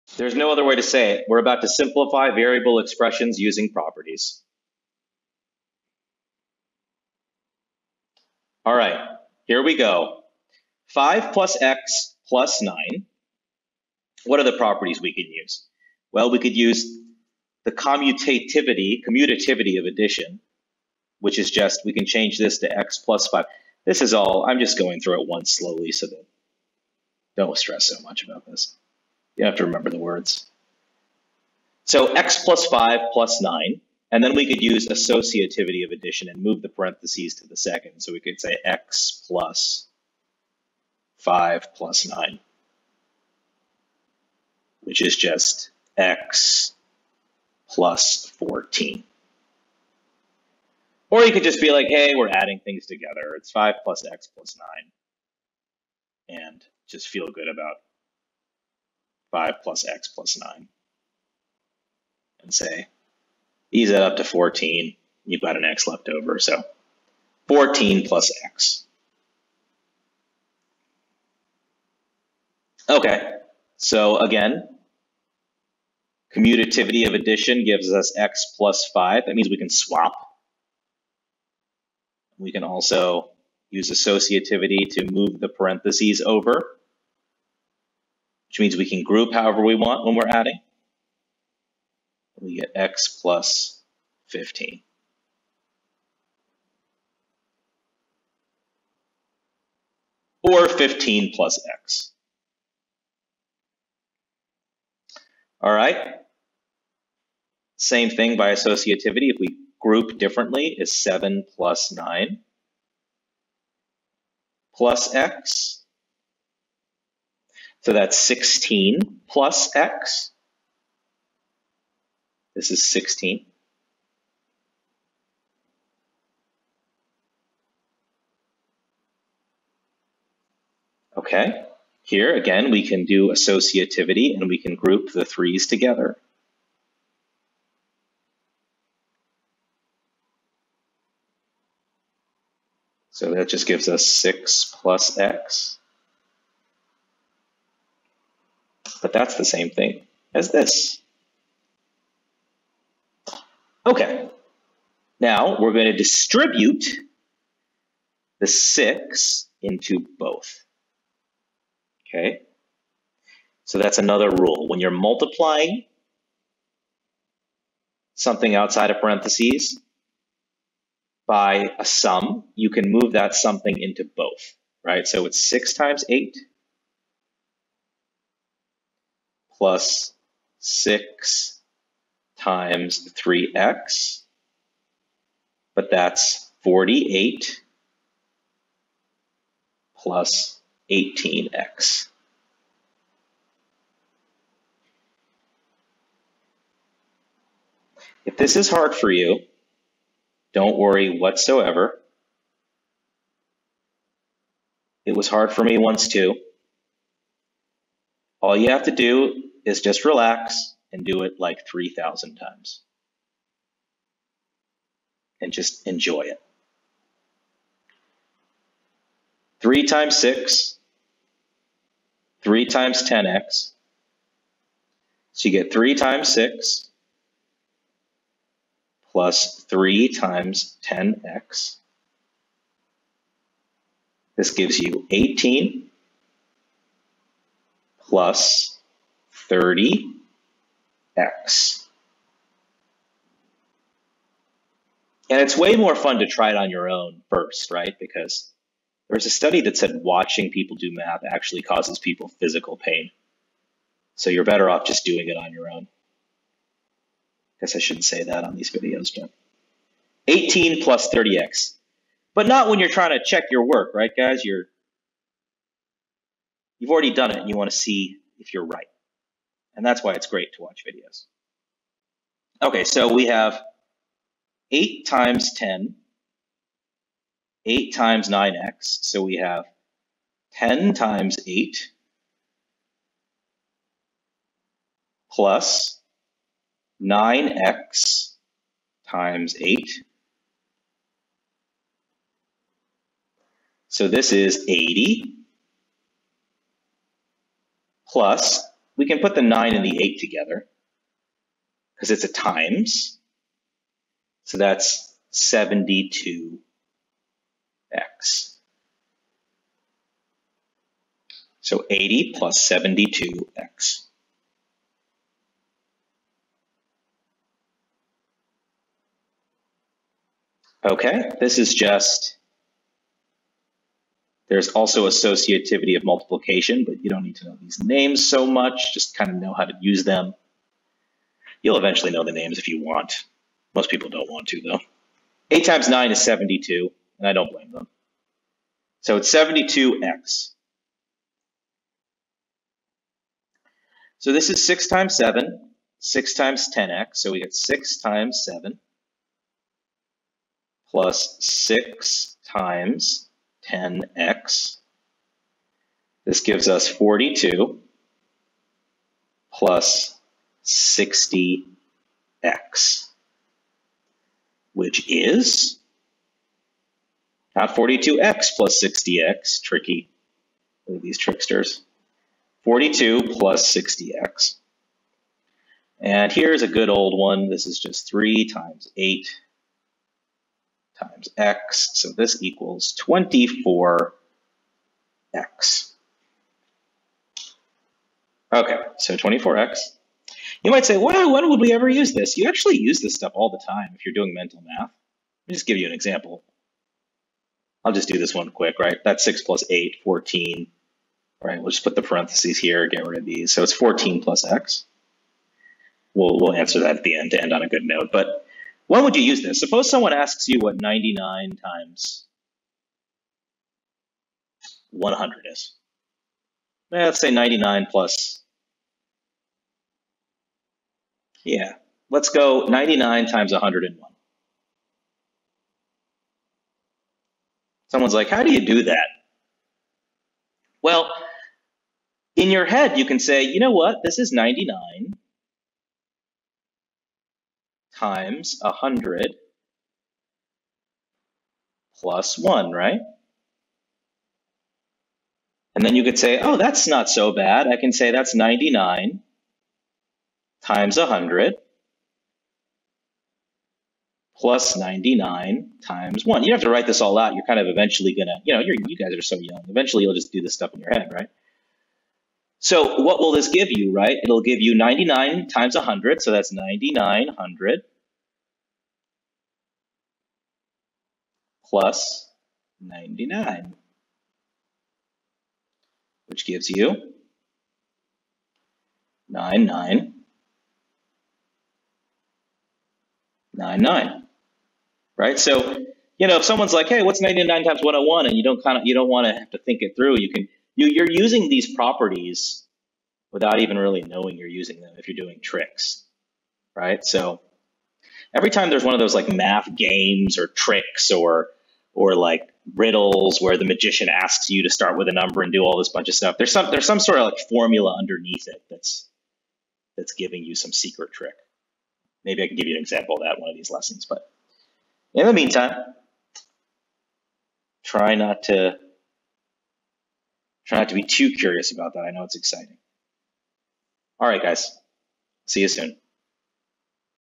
8.34 Simplify Variable Expressions Using Properties
Simplify Variable Expressions Using Properties
Practice simplifying variable expressions using properties.